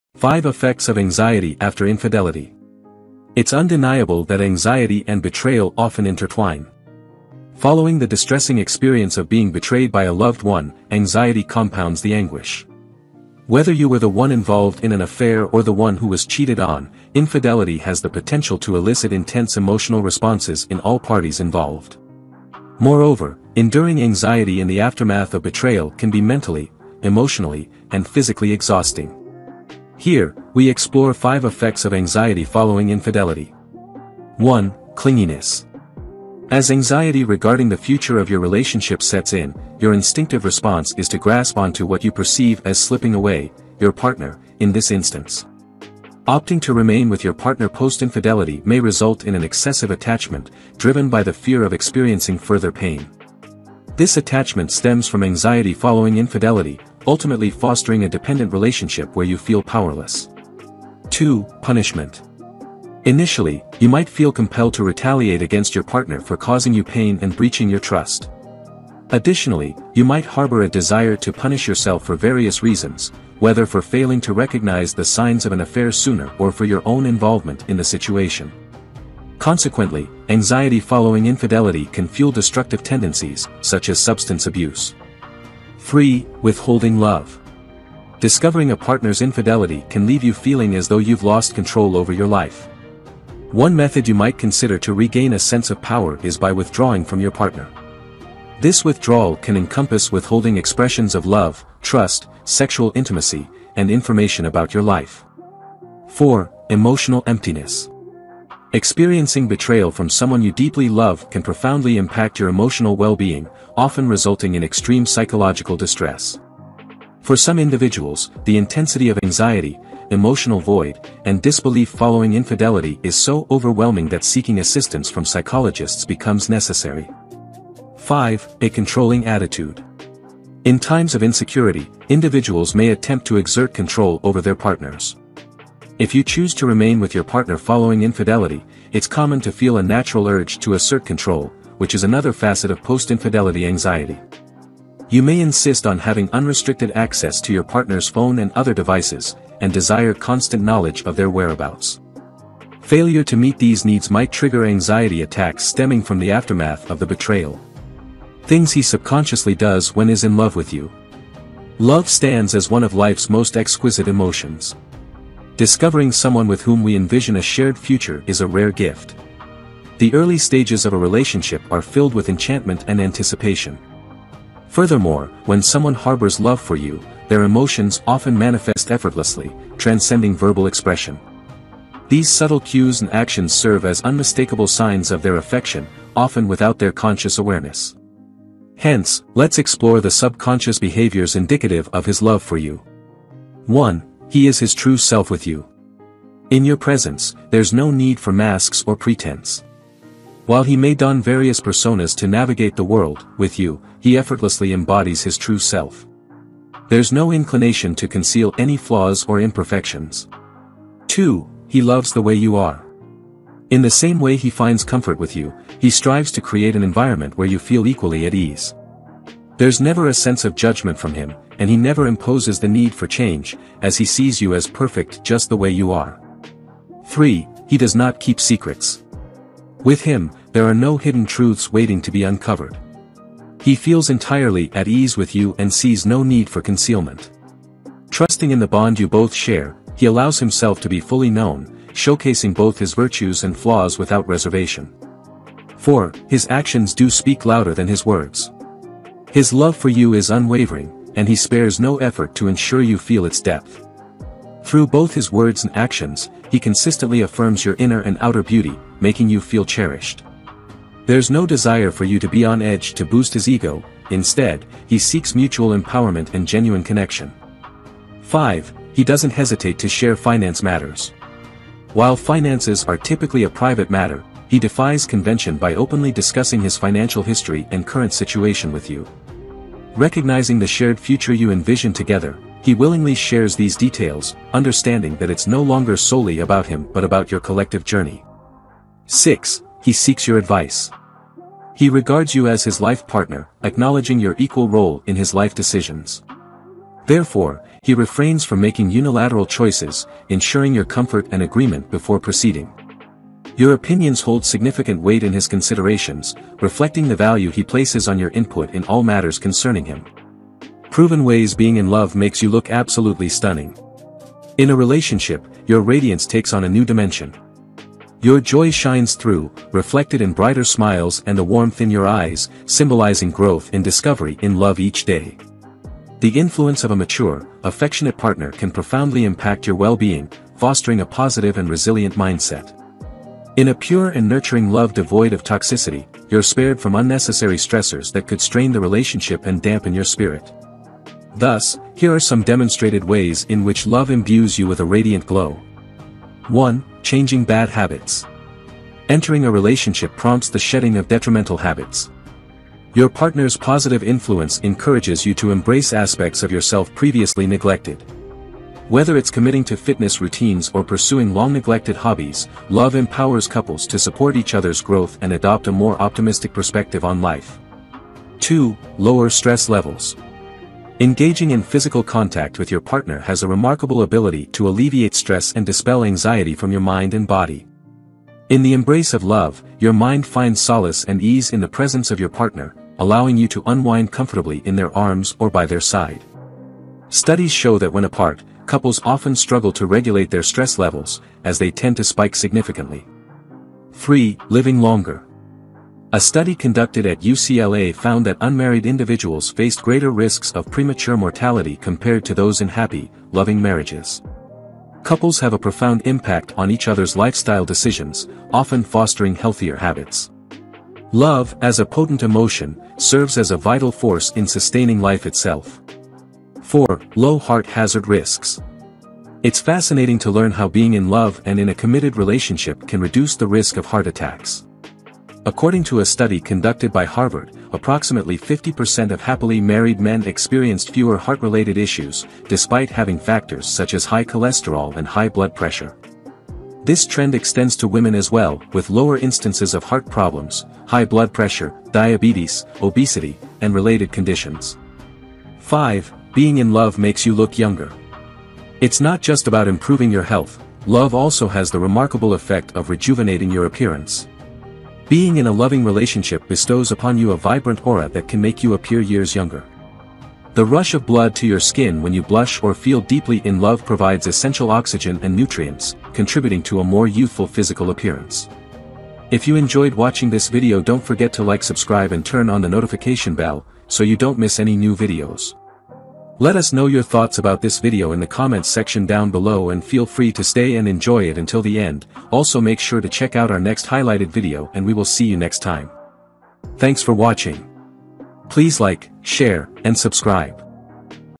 Things He Subconsciously Does When Is In Love With You | Human Behaviour Psychology | Super Facts
Love stands as one of life's most exquisite emotions. Discovering someone with whom we envision a shared future is a rare gift. The early stages of a relationship are filled with enchantment and anticipation. Furthermore, when someone harbors love for you, their emotions often manifest effortlessly, transcending verbal expression.
These subtle cues and actions serve as unmistakable signs of their affection, often without their conscious awareness. Hence, let's explore the subconscious behaviors indicative of his love for you.
1. He Is His True Self With You.
2. He Loves The Way You Are.
3. He Does Not Keep Secrets.
4. His Actions Do Speak Louder Than His Words.
5. He Doesn't Hesitate To Share Finance Matters.
6. He Seeks Your Advice.